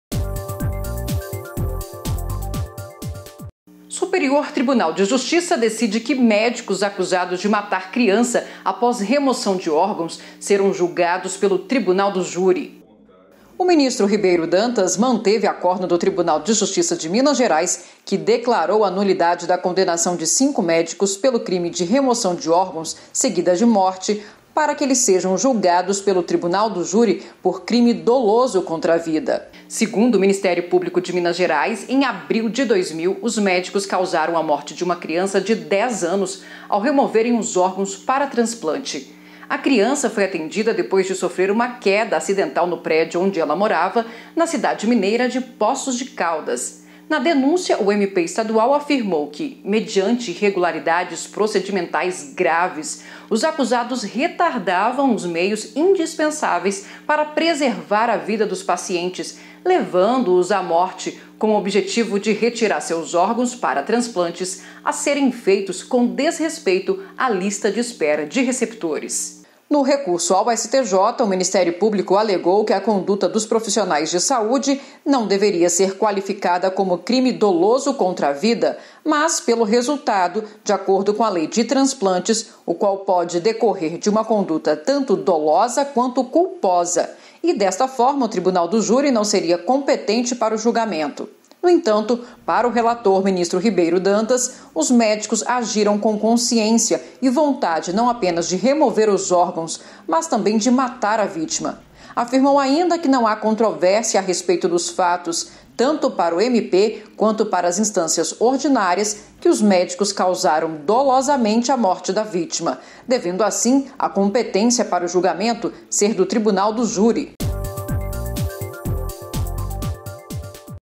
Médicos acusados de matar criança após remoção de órgãos serão julgados pelo tribunal do júri
​​O ministro do Superior Tribunal de Justiça (STJ) Ribeiro Dantas manteve acórdão do Tribunal de Justiça de Minas Gerais (TJMG) que declarou a nulidade da condenação de cinco médicos pelo crime de remoção de órgãos seguida de morte, para que eles sejam julgados pelo tribunal do júri por crime doloso contra a vida.